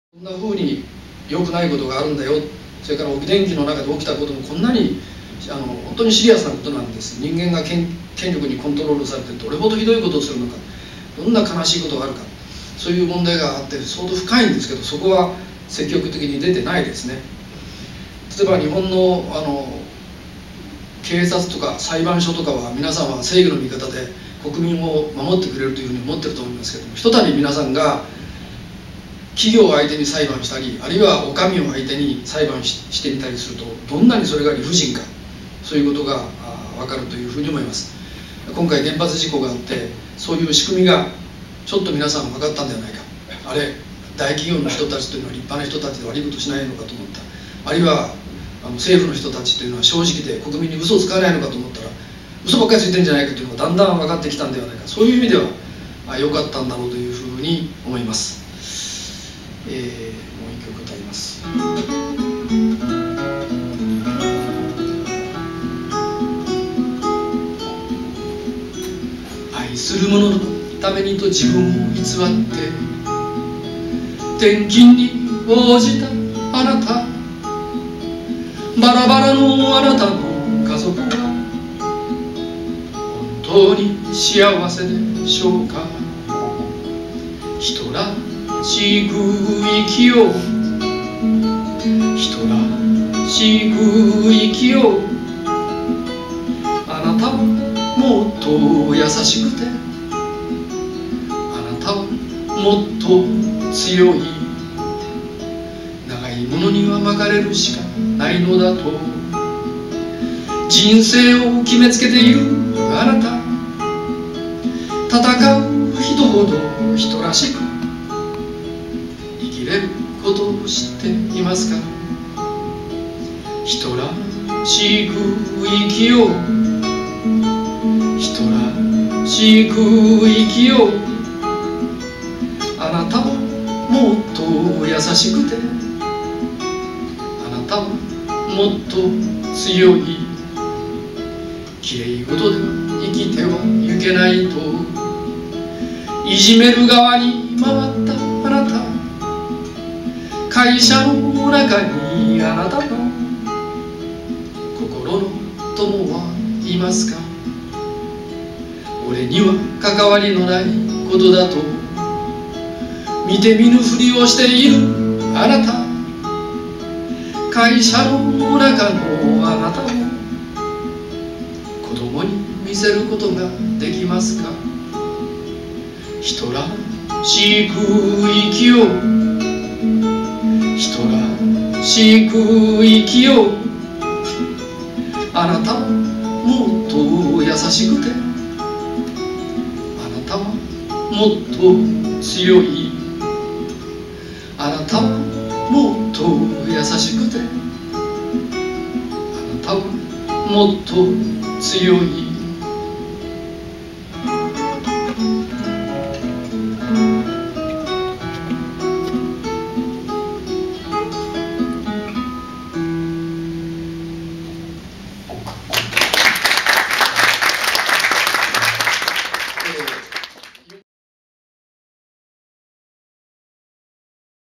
『田中さんはラジオ体操をしない』田中さんのナナゲイ・ミニライブ（2）
『田中さんはラジオ体操をしない』と言う映画を観に行ったら、田中哲朗さんの舞台挨拶 & ミニライブがあったと言う風景。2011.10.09